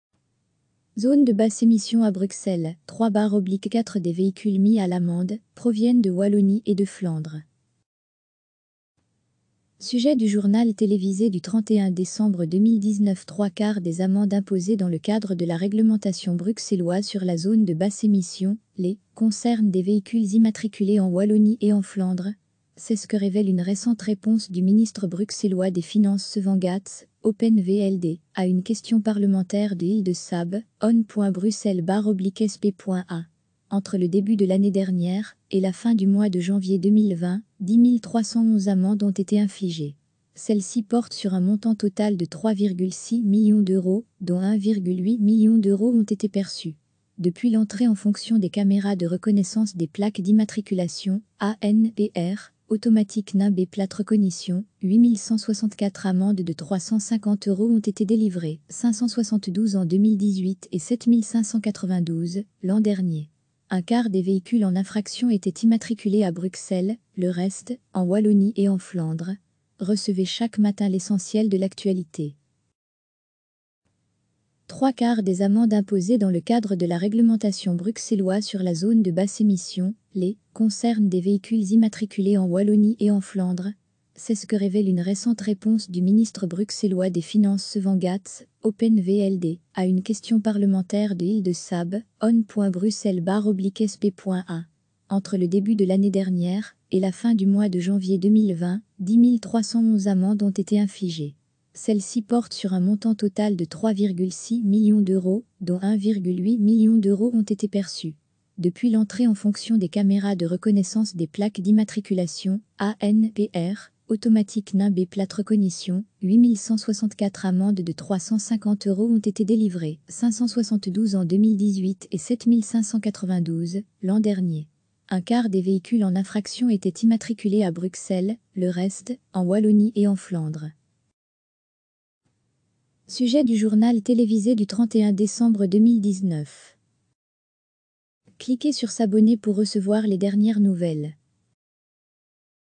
Zone de basse émission à Bruxelles: 3/4 des véhicules mis à l'amende proviennent de Wallonie et ...
Sujet du journal télévisé du 31/12/2019

Trois quarts des amendes imposées dans le cadre de la réglementation bruxelloise sur la zone de basse émission (LEZ) concernent des véhicules immatriculés en Wallonie et en Flandre. C'est ce que révèle une récente réponse du ministre bruxellois des Finances Svent Gatz (Open Vld) à une question parlementaire de Hilde Sabbe (one.brussels/sp.a).

Entre le début de l'année dernière et la fin du mois de janvier 2020, 10.311 amendes ont été infligées. Celles-ci portent sur un montant total de 3,6 millions d'euros, dont 1,8 million d'euros ont été perçus.

Depuis l'entrée en fonction des caméras de reconnaissance des plaques d'immatriculation (ANPR - Automatic Number Plate Recognition), 8164 amendes de 350 euros ont été délivrées: 572 en 2018 et 7592, l'an dernier. Un quart des véhicules en infraction étaient immatriculés à Bruxelles, le reste, en Wallonie et en Flandre.